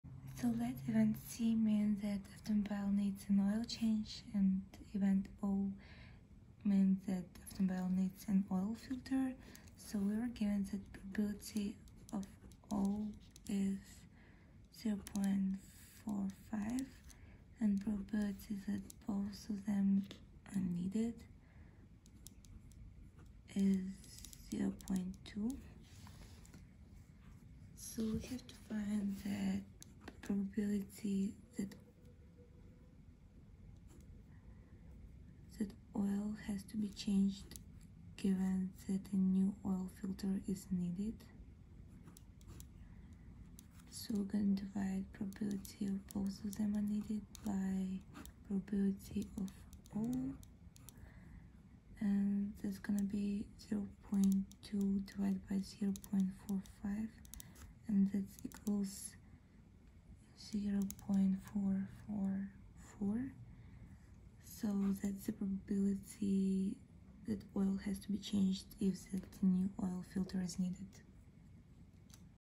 The probability that an automobile being filled with gasoline will also need an oil ... | Plainmath
Solution to Statistics and Probability question: The probability that an automobile being filled with gasoline will also need an oil change is 0.37; the probability that it needs a new oil filter is 0.45; and the probability that both the oil and filter need changing is 0.20.
If a new oil filter is needed, what is the probability that the oil has to be changed?